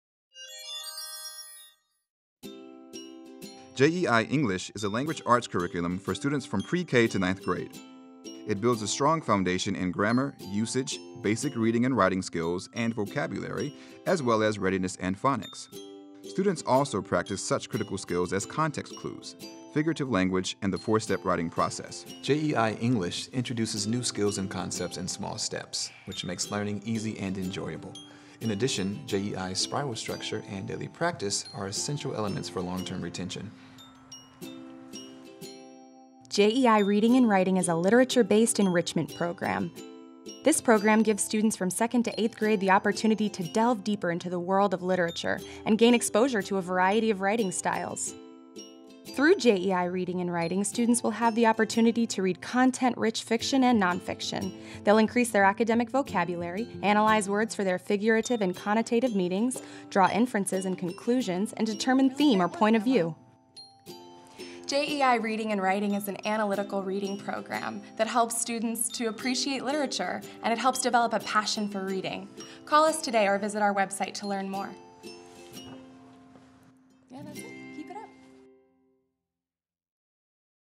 JEI English Program
JEI English is an enrichment learning program that goes beyond tutoring for students Pre-K and up. When your child starts learning with our personalized program they build a strong foundation in grammar, usage, basic reading and writing skills, and vocabulary as well as readiness and phonics. Students also practice
such critical skills as context clues, figurative language, and the 4-step writing process.